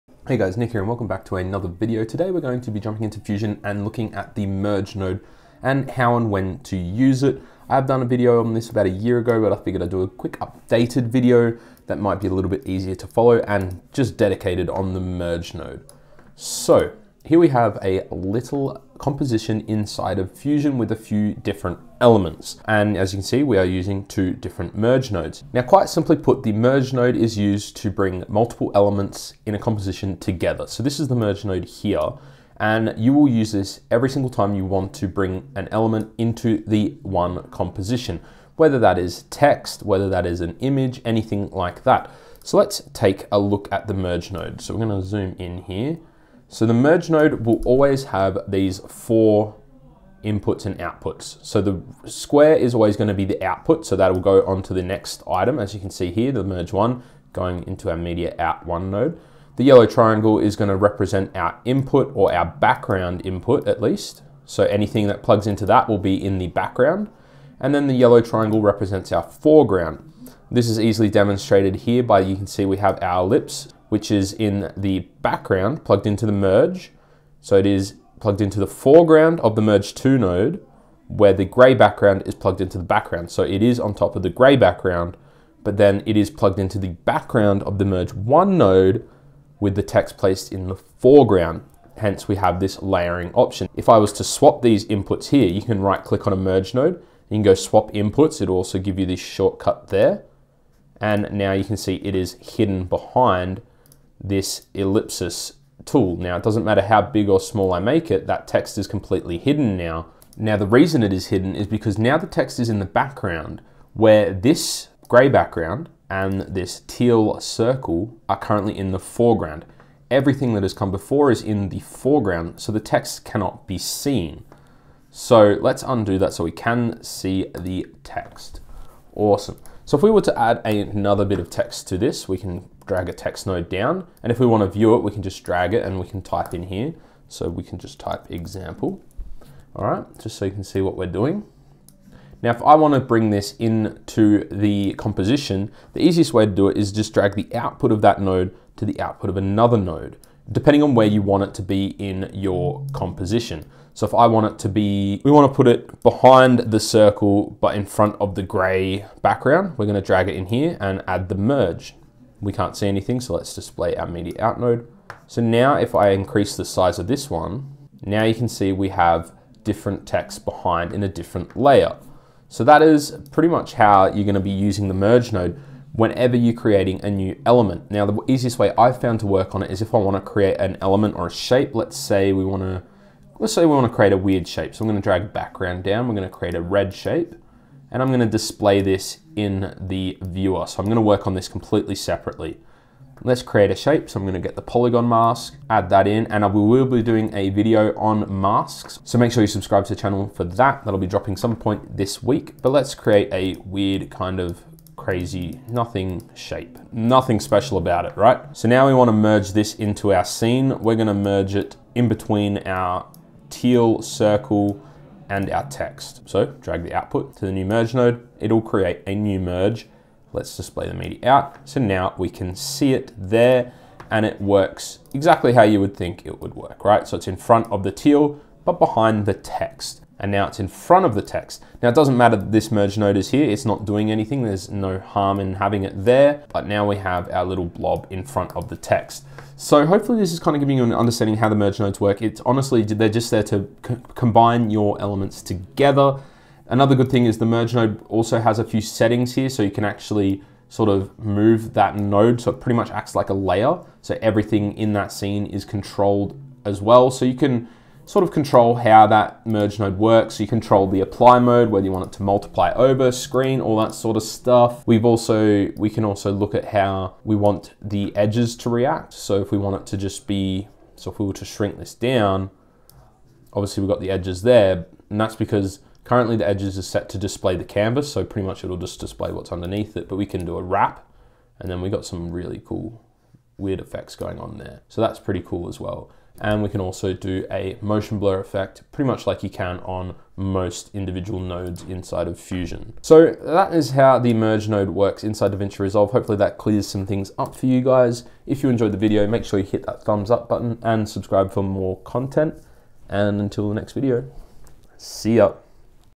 How to use the Merge Node in Davinci Resolve 16
In this video I will show you how to use the Merge node in Fusion inside of Davinci Resolve 16. The most common node you will use!